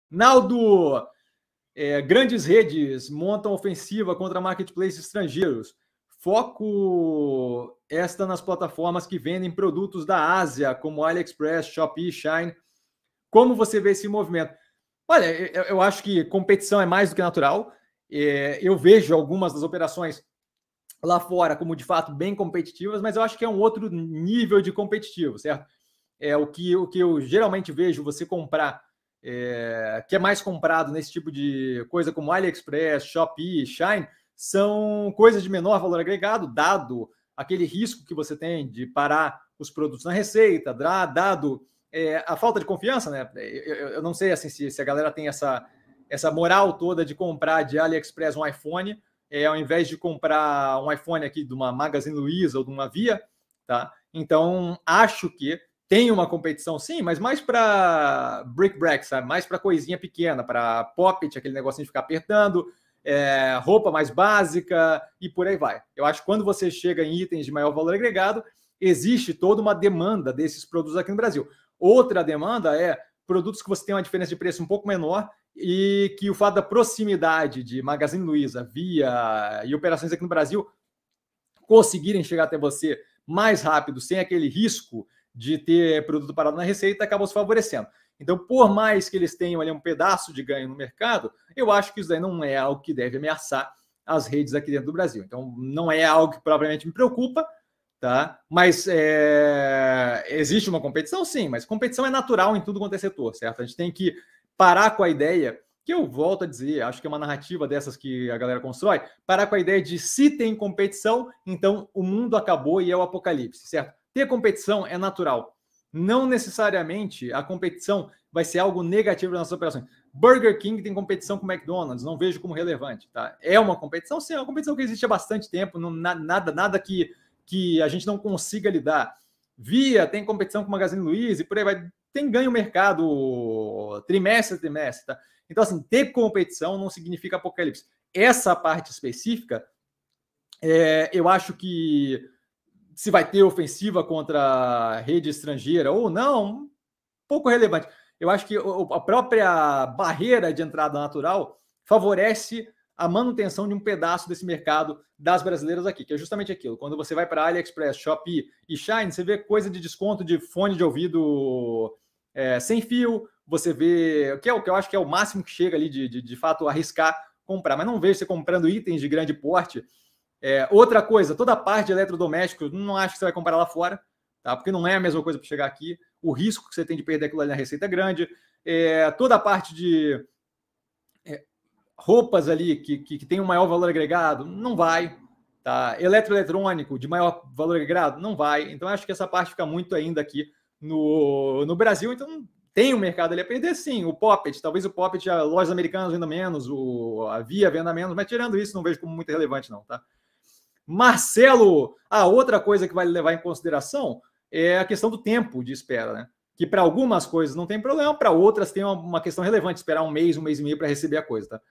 Como está a competição entre varejistas brasileiras e internacionais? - Live by Warren 021 19/02/22
Seleção das partes mais interessantes das Lives by Warren de segunda.